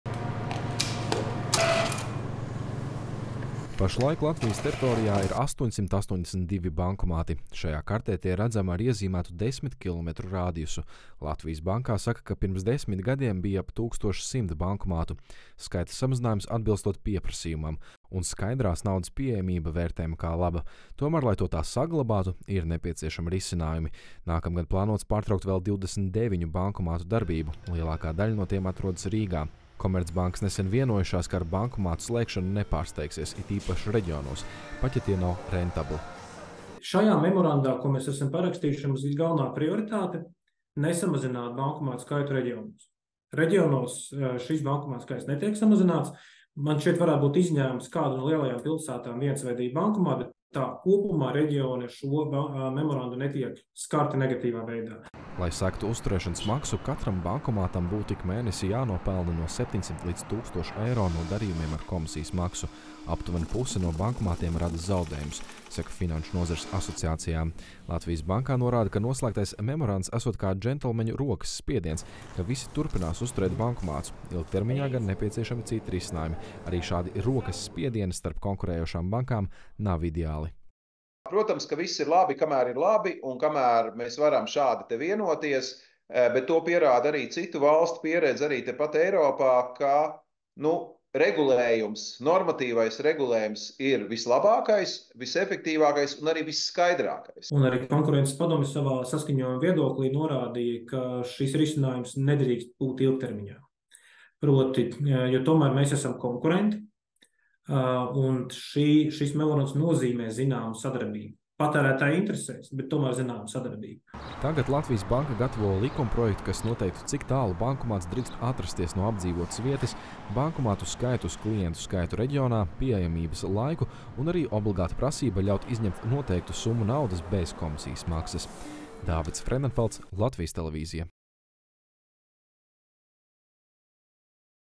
Bankomātu arvien mazāk; vienojas par skaidras naudas pieejamību
Latvijas Banka apņēmusies rosināt likuma izmaiņas, kas noteiktu par pienākumu bankām uzturēt bankomātus, lai skaidras naudas pieejamība tiktu nodrošināta visā valstī. Pēdējo desmit gadu laikā bankomātu kļūst arvien mazāk, to uzturēšana top dārgāka un sarūk arī klientu skaits. Tikmēr bankas savstarpēji vienojušās par pagaidu risinājumu.